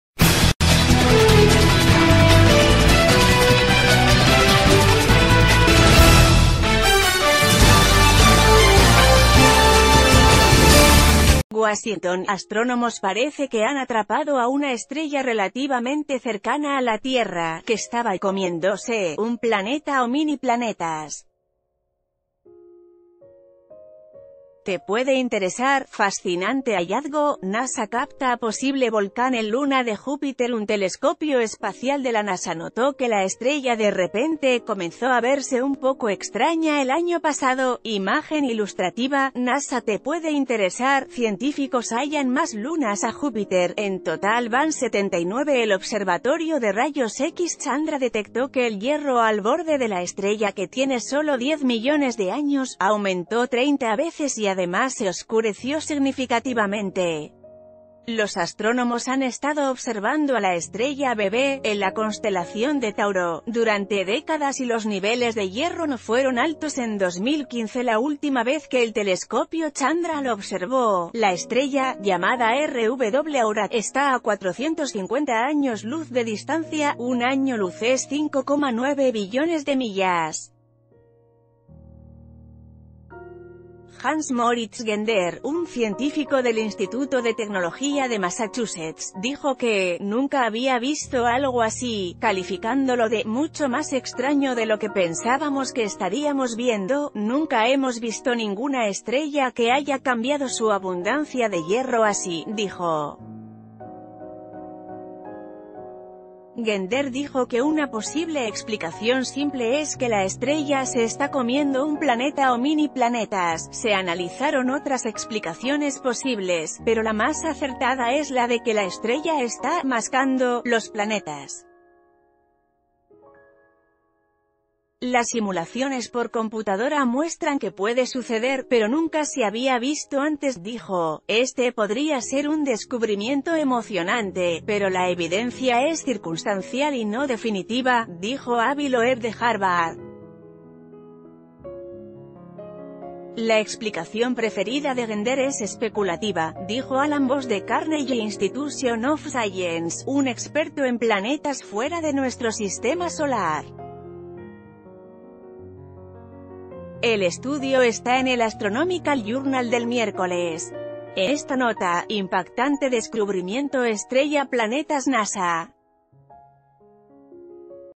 Insólito hallazgo de la NASA: captan estrella bebé que 'come' planetas
Washigton - Astrónomos parece que han atrapado a una estrella relativamente cercana a la Tierra, que estaba 'comiendóse' un planeta o mini planetas. Te puede interesar: Fascinante hallazgo: NASA capta posible volcán en luna de JúpiterUn telescopio espacial de la NASA notó que la estrella de repente ...